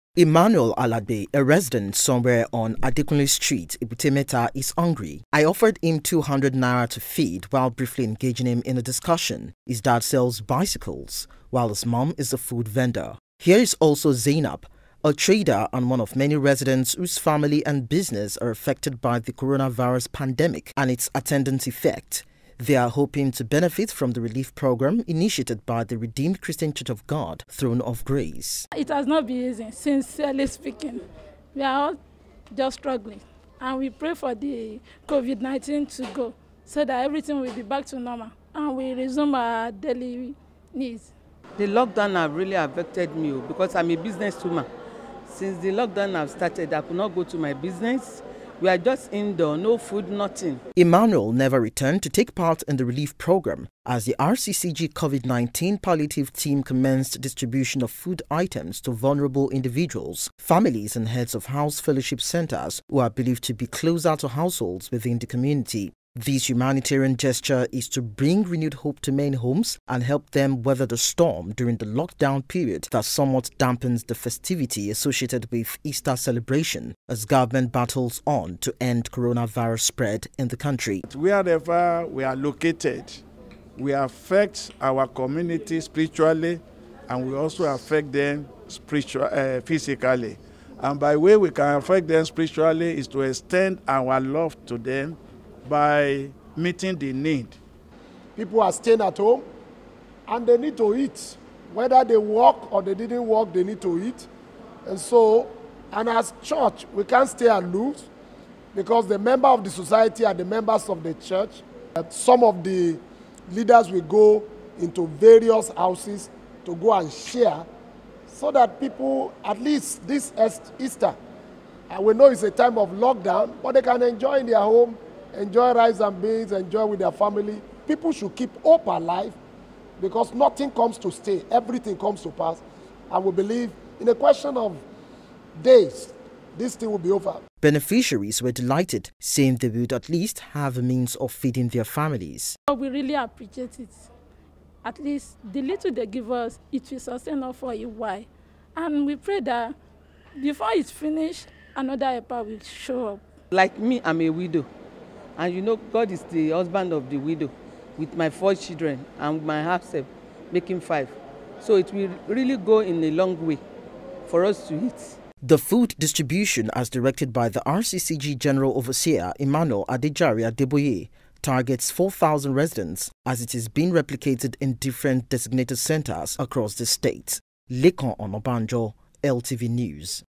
His Love Foundation Distributed Relief Package
The General Overseer of The Redeemed Christian Church of God, Pastor E.A. Adeboye gave the directive for all RCCG parishes to distribute food items to both members and non-members of the Church. This initiative is to help provide support to those who need food items during this lock down period.